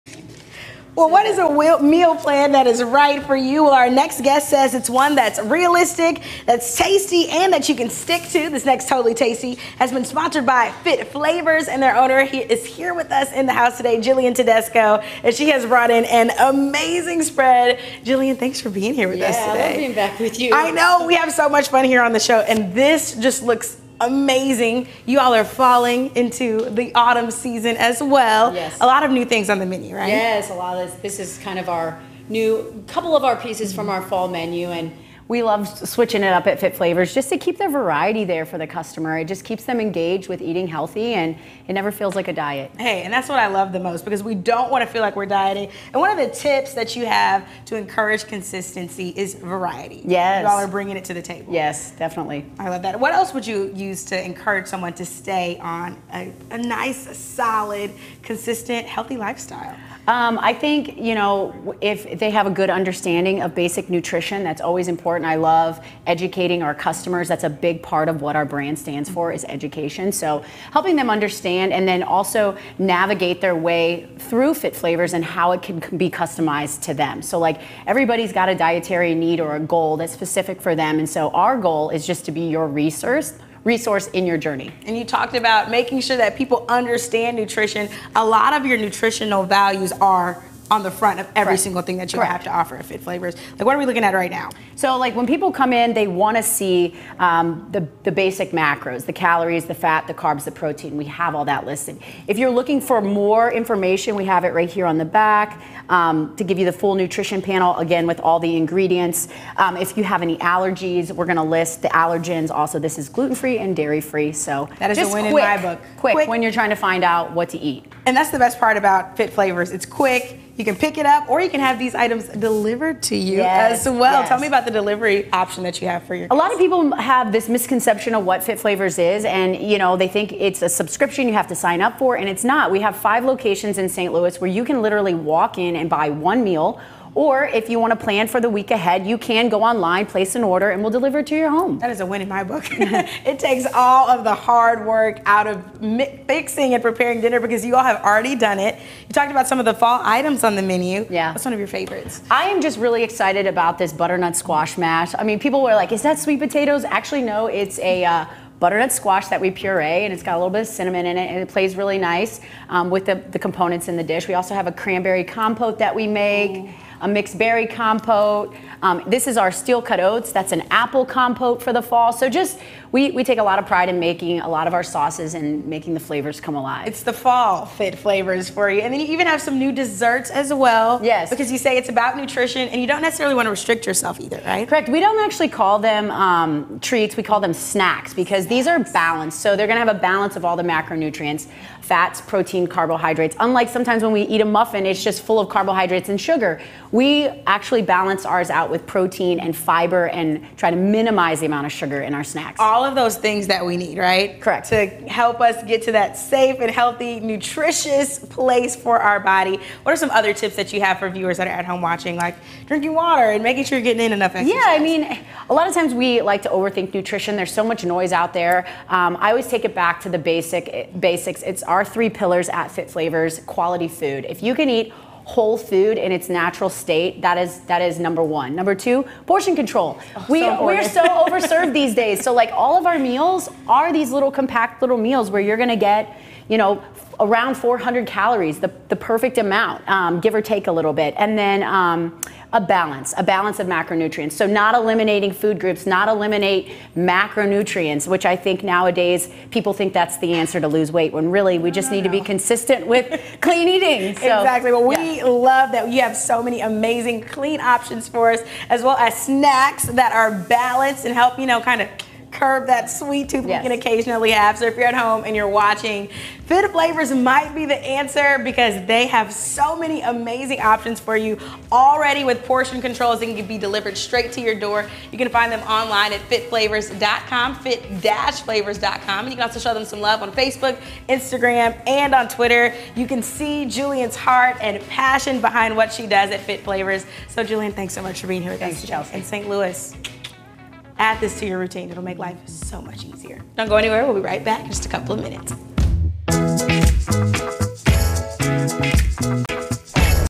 Fit-Flavors serves up totally tasty
One of the mottos at Fit-Flavors is everyone has to love what they are eating, and if they don't, then stop eating it.